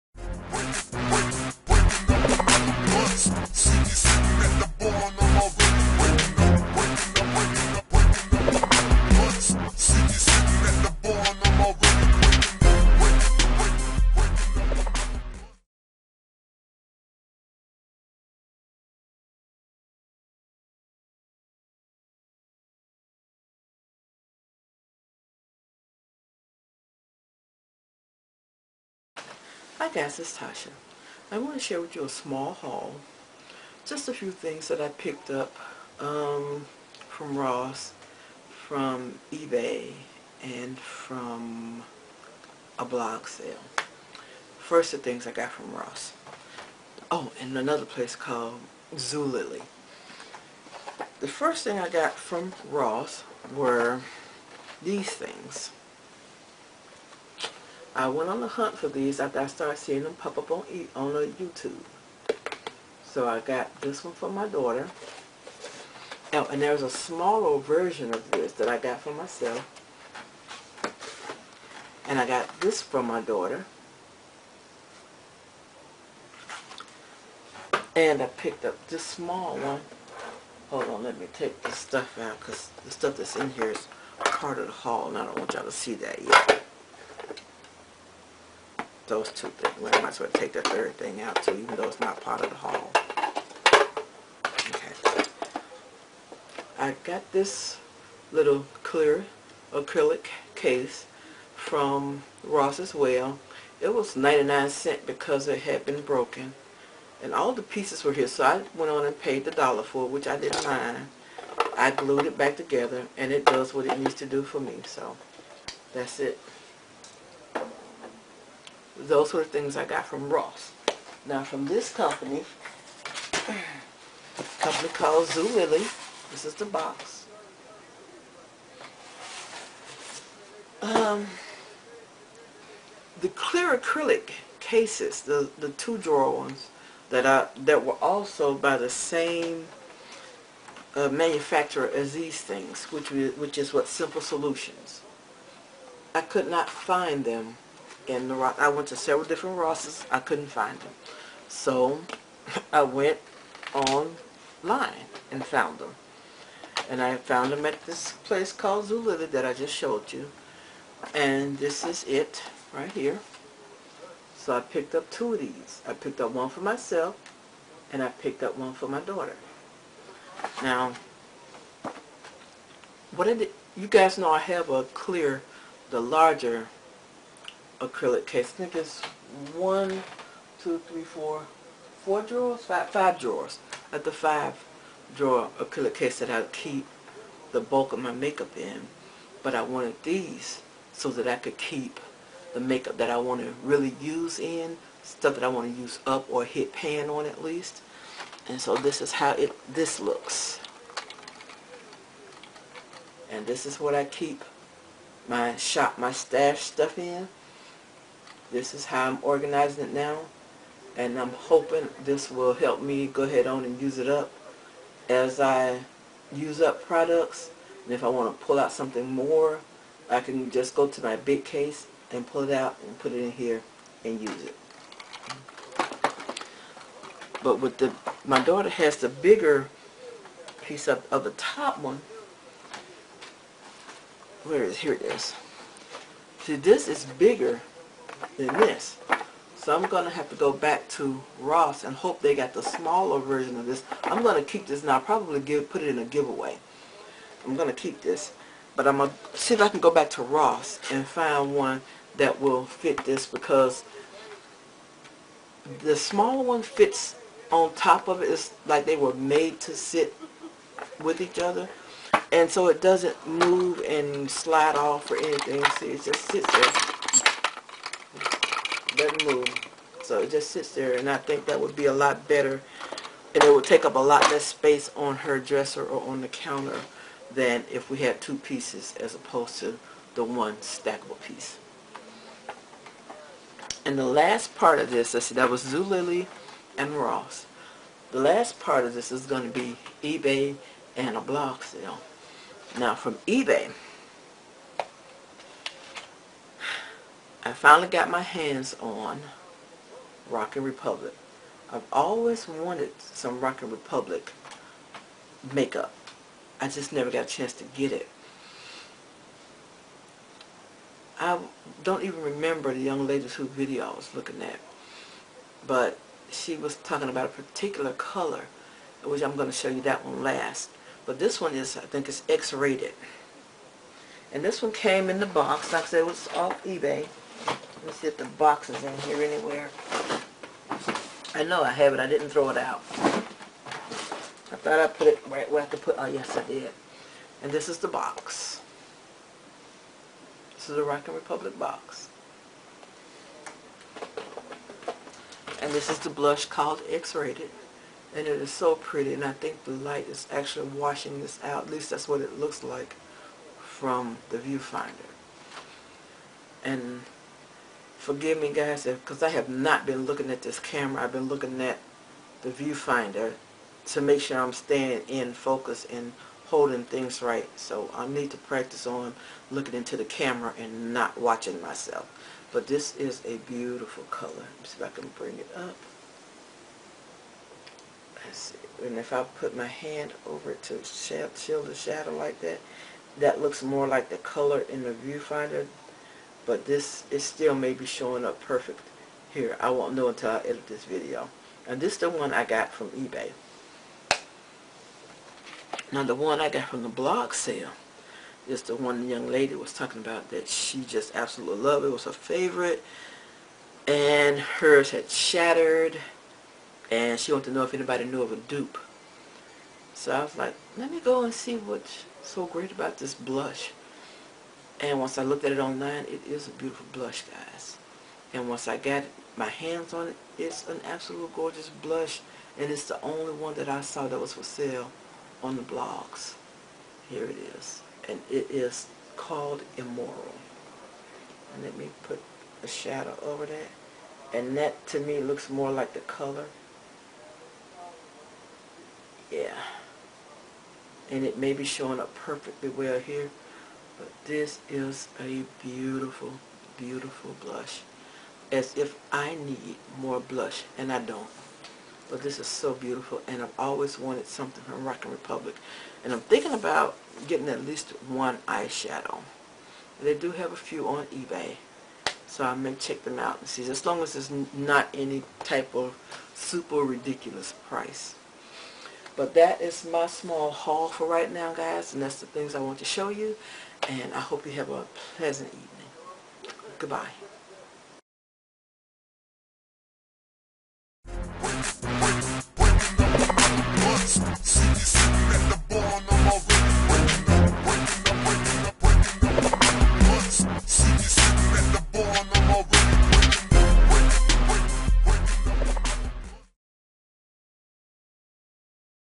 Small Collective Haulage
Tracy Timberlake is the young lady that is looking for a dupe of Rock & Republic Blush in the color Immoral.  The link below will take you to the video that am referring to in the video.